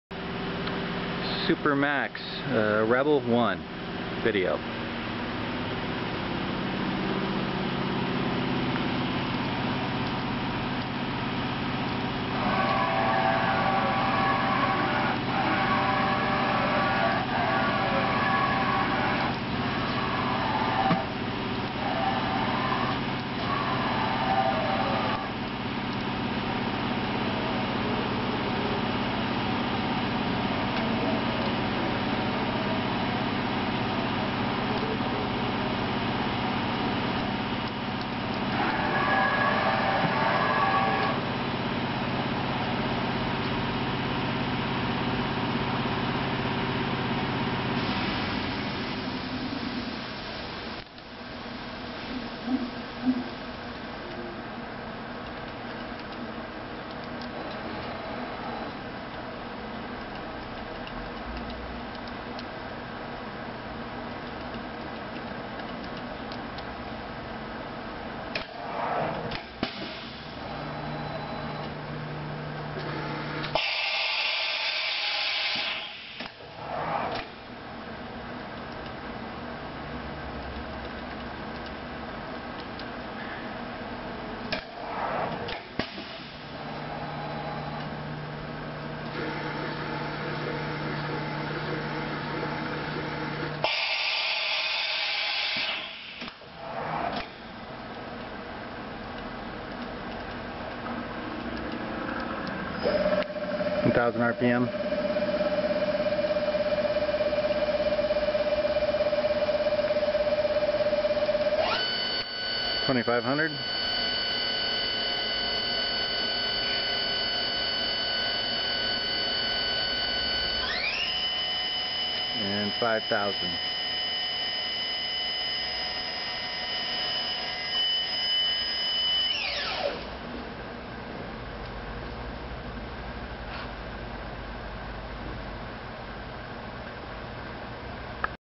338864 1998 Supermax Rebel 1
General Specifications As Per Brochure (Please Verify Details):

Machine Travels (X/Y/Z):                       25.5" x 16.1" x 18.1"
Spindle To Table Distance:                     3.94" - 22.05"
Spindle Center to Column Distance:             14.76"
Table Capacity:                                770 Lbs.
Rapid Traverse Rate (X/Y/Z):                   472 / 472 / 394 IPM
Maximum Cutting Feed Rate:                     1 - 119 IPM
Tool Changer Style:                            Side-Mount, Twin arm
Tool Changer Capacity:                         20 Stations
Spindle Drive:                                 15 / 10 HP

Equipped With:
  Fanuc 0M CNC Control
  20 Station Tool Changer
  Hand Wheel

Dimensions:  86" x 93" x 110" 8,000 lbs
Tank:  74" x 78" x 13" 150 lbs